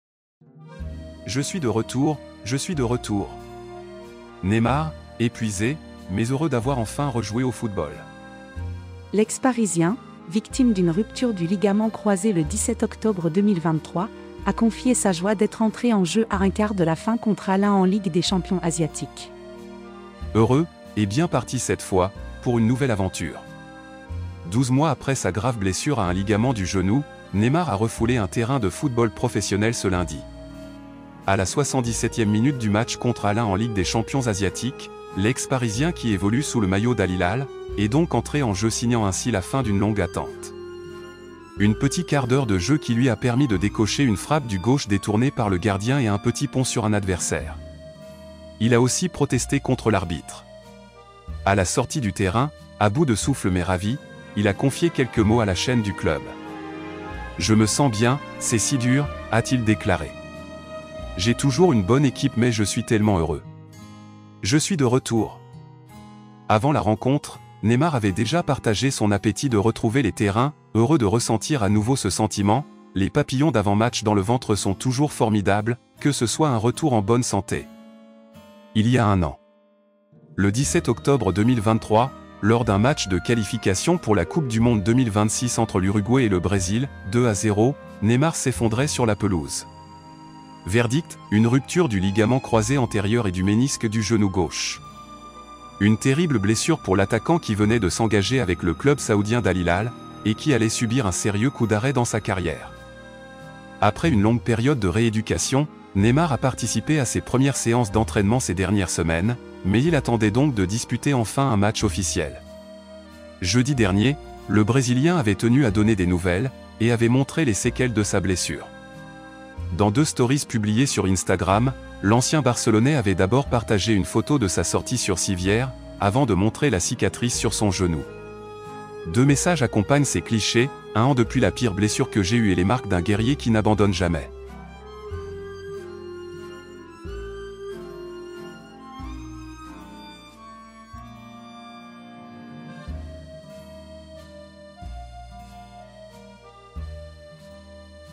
« Je suis de retour, je suis de retour ! » : Neymar, épuisé, mais heureux d’avoir enfin rejoué au...
« Je suis de retour, je suis de retour ! » : Neymar, épuisé, mais heureux d’avoir enfin rejoué au football

L’ex-Parisien, victime d’une rupture du ligament croisé le 17 octobre 2023, a confié sa joie d’être entré en jeu à un quart de la fin contre Al-Ain en Ligue des champions asiatique.

Par Le Parisien